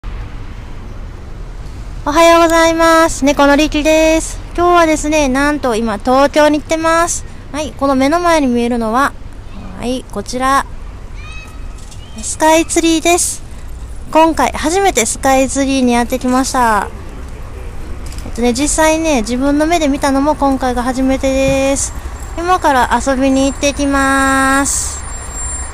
東京スカイツリー見に来たよ！【東京観光 旅行 】 Tokyo Skytree ソラマチ
東京スカイツリーにやってきました！
遠目からの撮影です。

ouTube Captureから